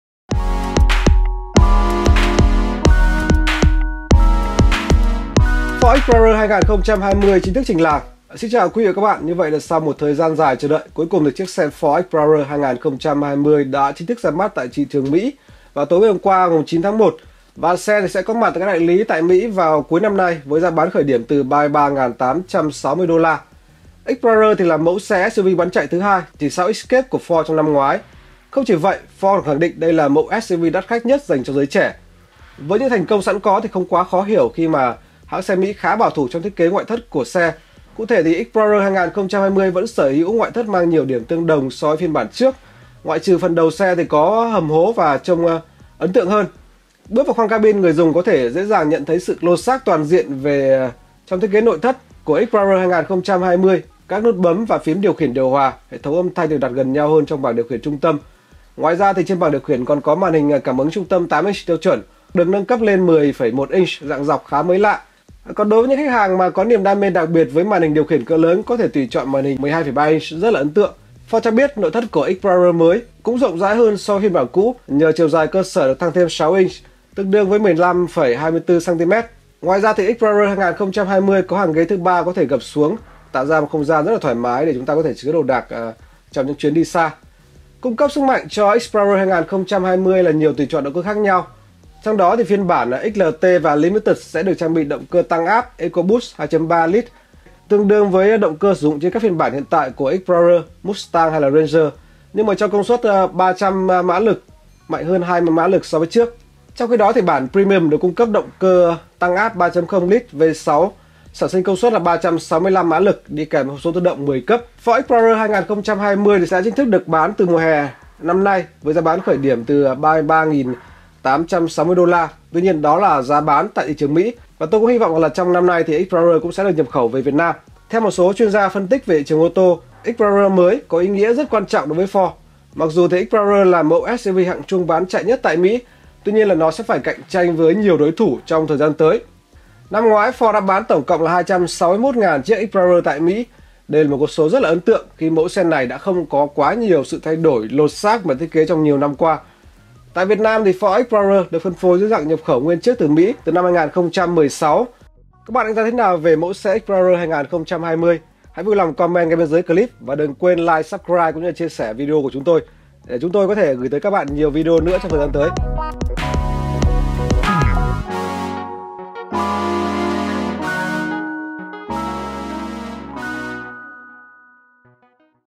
Ford Explorer 2020 trình làng: "ĐẦU GẤU" hơn, màn hình cảm ứng dọc, HÓNG ngày về Việt Nam
Hãng xe Ford vừa chính thức trình làng mẫu SUV Explorer 2020 hoàn toàn mới vào tối 9/1. Xe sẽ có mặt tại các đại lý vào cuối năm nay với giá khởi điểm từ 33.860 USD.

Explorer là mẫu SUV bán chạy thứ hai (chỉ sau mẫu Escape) của Ford trong năm ngoái. Không chỉ vậy, Ford còn khẳng định đây là mẫu SUV đắt khách nhất dành cho giới trẻ. Với những thành công có sẵn, không quá khó hiểu khi hãng xe Mỹ khá “bảo thủ” trong thiết kế ngoại thất của xe. Cụ thể,Ford Explorer 2020 vẫn sở hữu ngoại thất mang nhiều điểm tương đồng của phiên bản tiền nhiệm, ngoại trừ mũi xe hầm hố hơn với với lưới tản nhiệt cỡ lớn, đường gờ hông và trần xe thể thao hơn.

Bước vào cabin, người dùng dễ dàng nhận thấy sự lột xác toàn diện trong thiết kế nội thất của Explorer 2020. Các nút bấm và phím điều khiển điều hòa, hệ thống âm thanh được đặt gần nhau hơn trong bảng điều khiển trung tâm. Ngoài ra, trên bảng điều khiển còn có màn hình cảm ứng trung tâm 8 inch tiêu chuẩn hoặc được nâng cấp lên màn hình 10,1 inch dạng dọc khá mới lạ. Đối với những khách hàng có niềm đam mê đặc biệt với màn hình điều khiển cỡ lớn có thể lựa chọn tùy chọn màn hình 12,3 inch.

Ford cho biết nội thất của Explorer mới cũng rộng rãi hơn so với phiên bản cũ nhờ chiều dài cơ sở được tăng lên 6 inch (15,24 cm). Bên cạnh đó, Ford Explorer 2020 có hàng ghế thứ ba có thể gập xuống, tạo ra một không gian thoải mái để chất đồ đạc.

Cung cấp sức mạnh cho Explorer 2020 là các động cơ khác nhau. Cụ thể, phiên bản XLT và Limited sẽ được trang bị động cơ tăng áp 2.3L, 4 xy lanh, tương tự như động cơ được sử dụng trên phiên bản hiện tại của Explorer, Mustang và Ranger, nhưng cho công suất tối đa 300 mã lực, mạnh hơn 20 mã lực so với người tiền nhiệm.

Trong khi đó, bản Premium được cung cấp động cơ tăng áp kép 3.0L, V6 sản sinh công suất 365 mã lực, đi kèm hộp số tự động 10 cấp.

Explorer 2020 sẽ chính thức được mở bán từ mùa hè năm 2019 với giá khởi điểm từ 33.860 USD.

Trả lời phỏng vấn của CNBC, Jeff Schuster, chuyên gia phân tích tại LMC Automotive, đánh giá Explorer mới có ý nghĩa rất quan trọng đối với Ford. Mặc dù Explorer là chiếc SUV hạng trung bán chạy nhất tại Mỹ, nhưng sẽ nó sẽ phải đối mặt với nhiều sự cạnh tranh từ các mẫu xe mới sẽ xuất hiện trong vài năm tới.